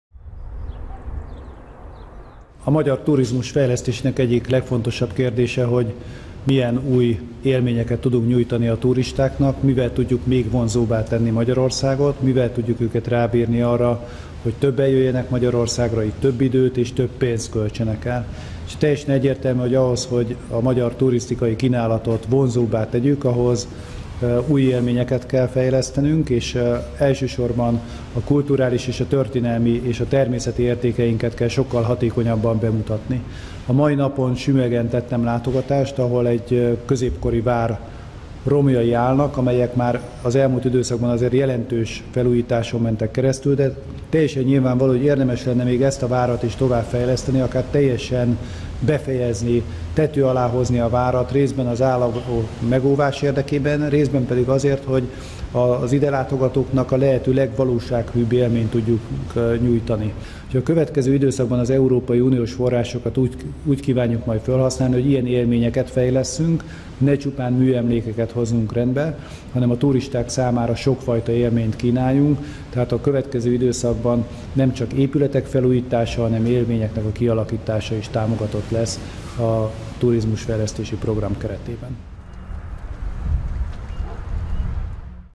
Glattfelder Béla a sümegi fejlesztésekről tájékozódott
Glattfelder Béla gazdaságszabályozásért felelős államtitkár sümegi látogatása során a tavalyi év végén lezárult programok eredményeiről, és a sümegi vár jövőbeli fejlesztési terveiről tájékozódott. Sümeg turisztikai vonzereje az elmúlt négy évben jelentősen nőtt, a kereskedelmi szálláshelyek vendégforgalma 68%-kal emelkedett.